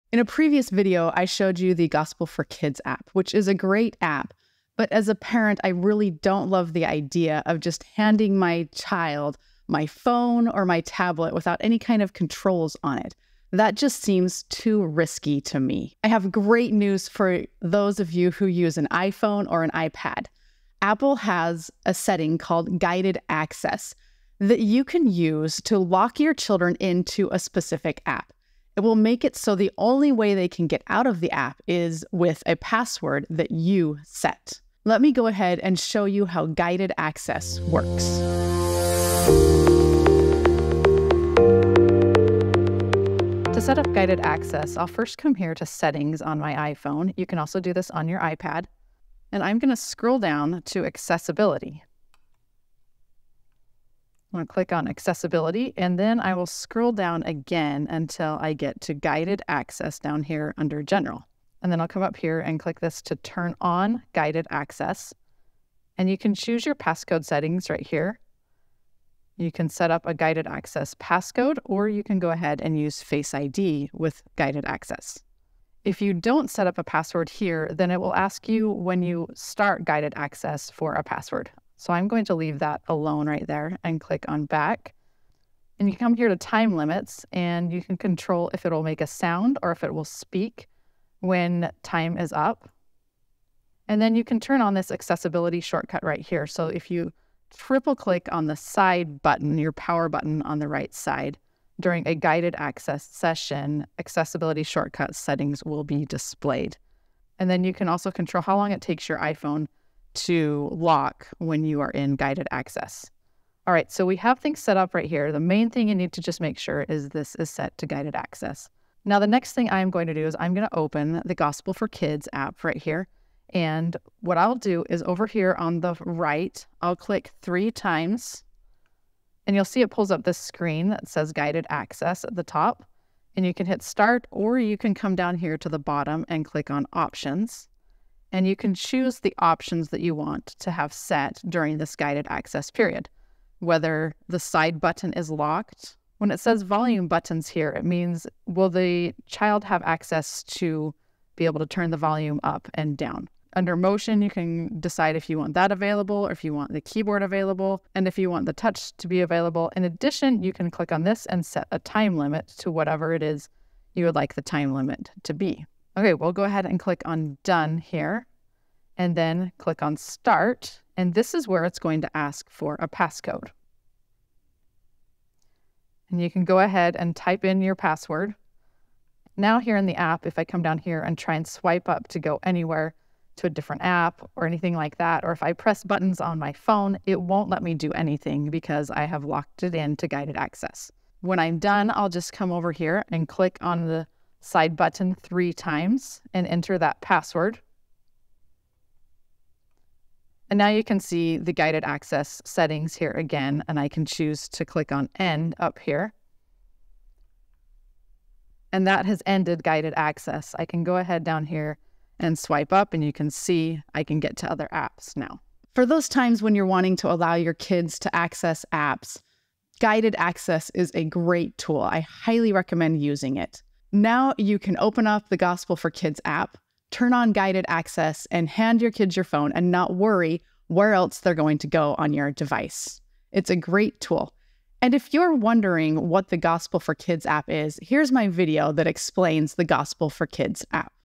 How to use guided access to keep your kids safe on an iphone or ipad
Keep your kids safe by locking them into an app before you hand them an iphone or ipad. This video is all about how to turn on guided access on an iphone or ipad.